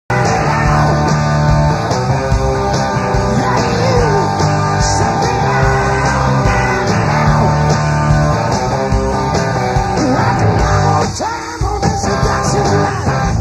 shake those gorgeous Basset ears
Basset Hound,  big ears are used to waft smells, Bassets are blood hounds bred with smaller legs for small prey, a working breed but hard to train. Also called scent hounds.